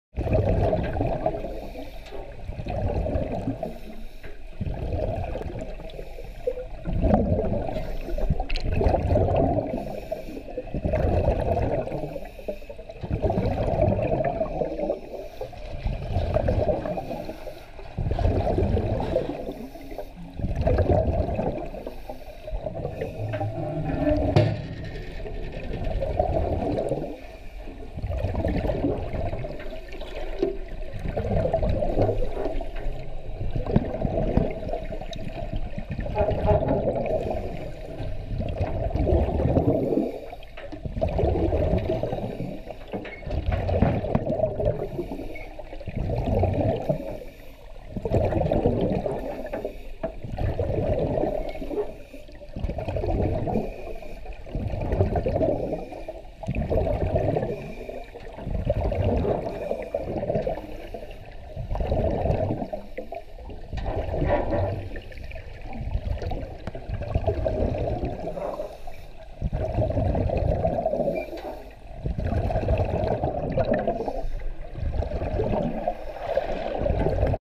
Shark cage diving 4th January 2018
Guest Matt Flint filmed this footage during dive on Thursday's charter. On this day we saw 4 different sharks!